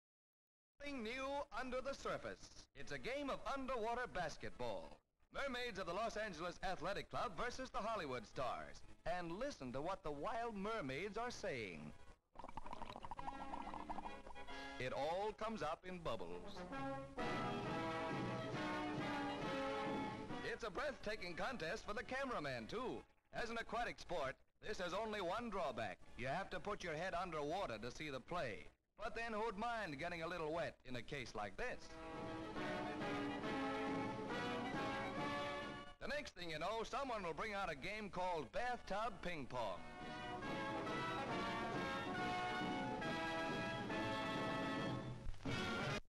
Girls Play Underwater Basketball (1934)
Town House, Los Angeles, California, United States of America (USA).

Young women dive into an outdoor swimming pool.

VS of the girls playing a strange underwater game that is closely liked with basketball.

BV of cameraman taking film through window in side of pool.

More shots of game being played.
 FILM ID:520.2